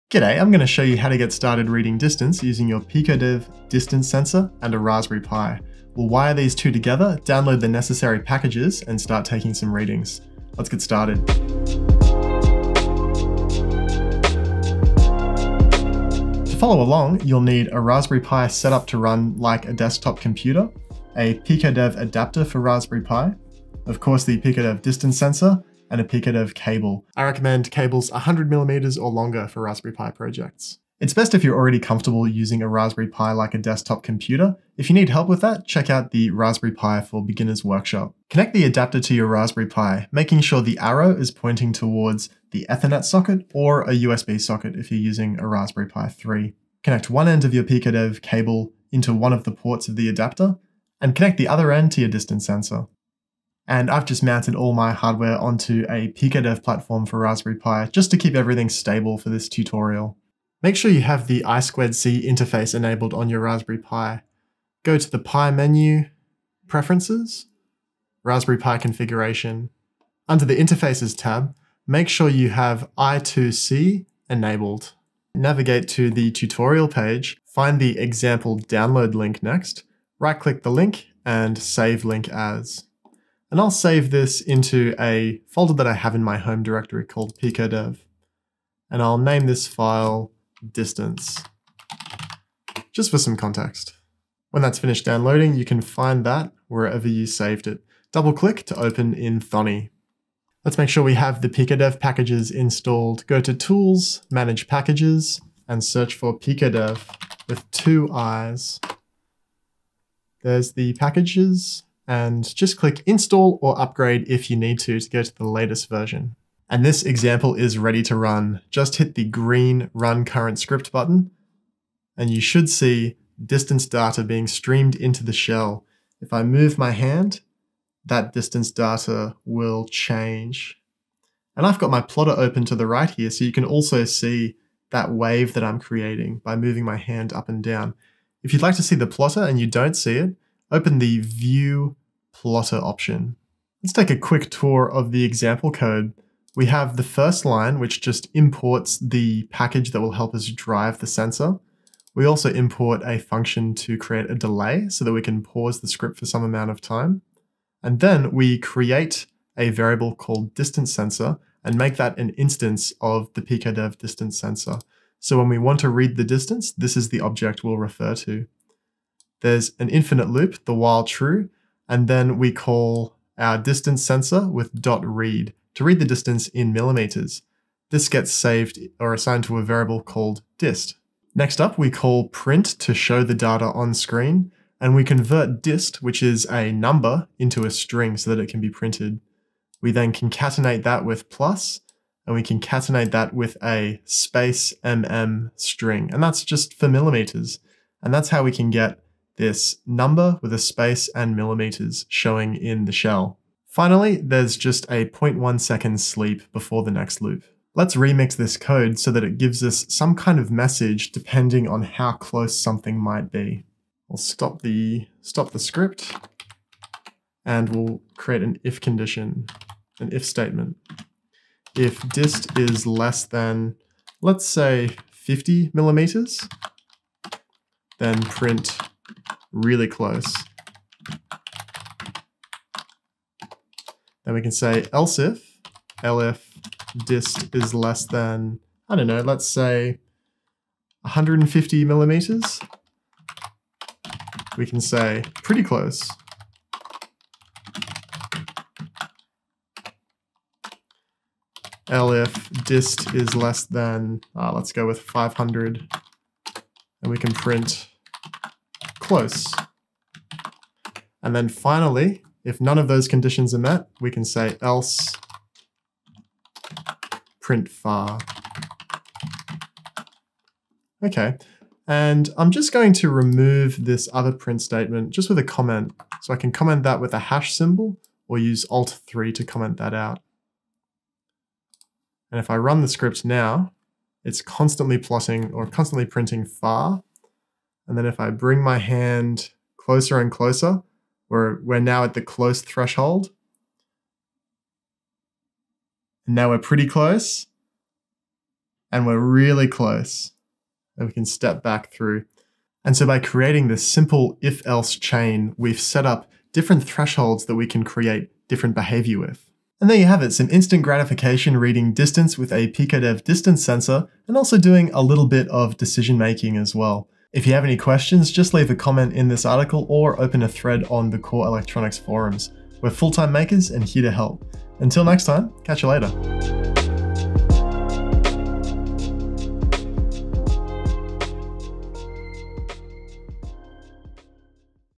PiicoDev Distance Sensor VL53L1X - Raspberry Pi Guide
This tutorial will guide you through wiring, uploading example MicroPython code in Thonny, and remixing the example to include new functionality.

The PiicoDev Distance Sensor uses a VL53L1X which can measure indoor ranges up to about 4m with 1% repeatability!

"PiicoDev" and the PiicoDev Logo are trademarks of Core Electronics Pty Ltd.